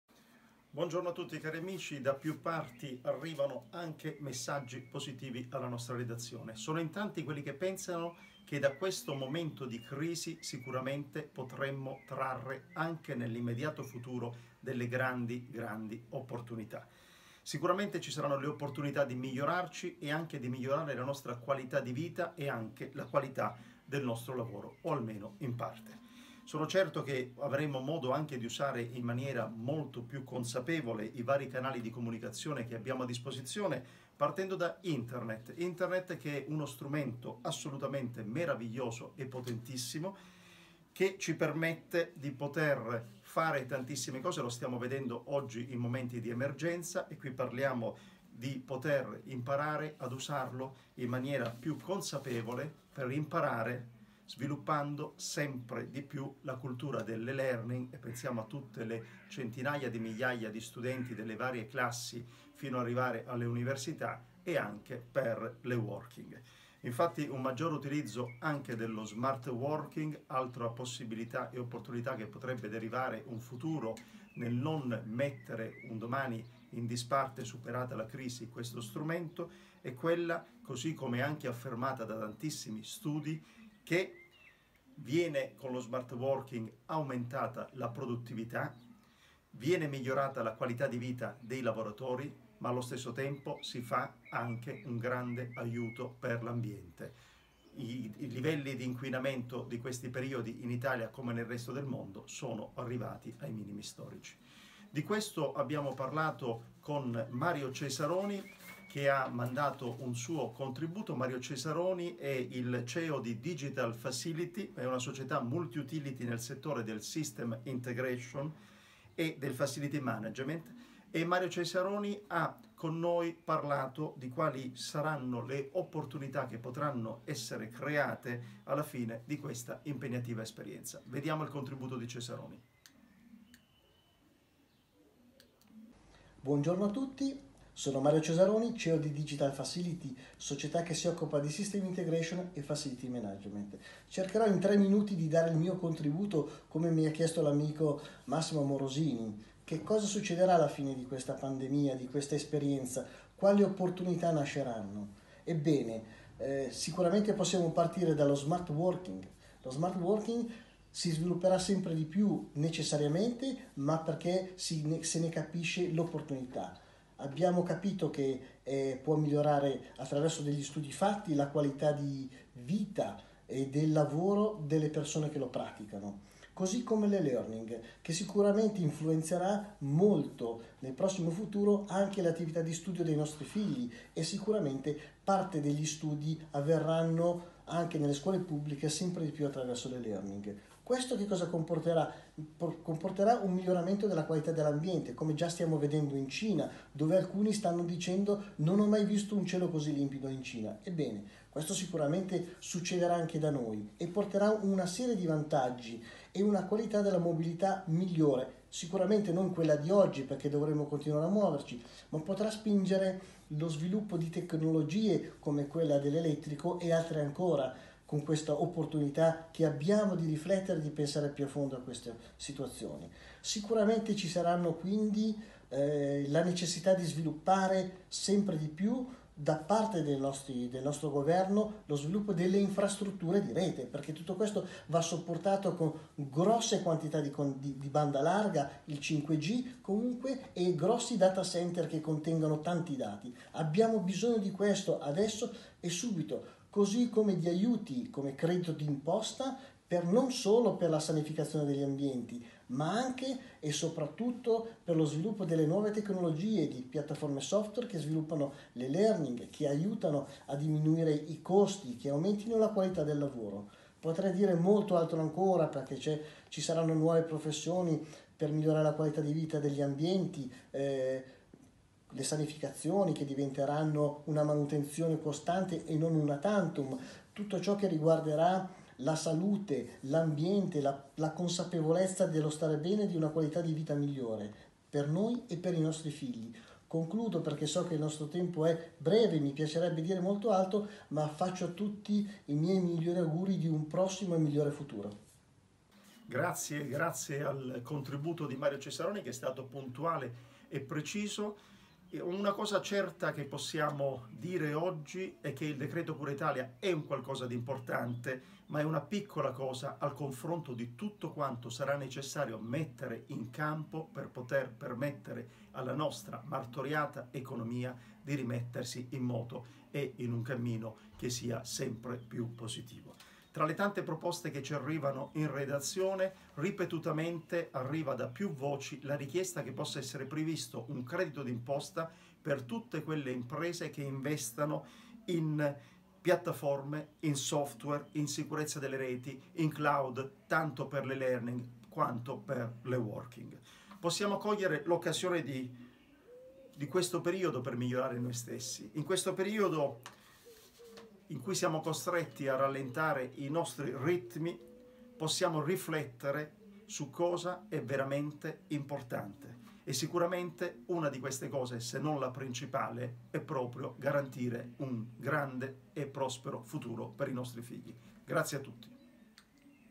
e-learning e e-working nuove opportunità
Learning e-working nuove opportunità
intervista a Mario Cesaroni imprenditore CEO di Digital Facility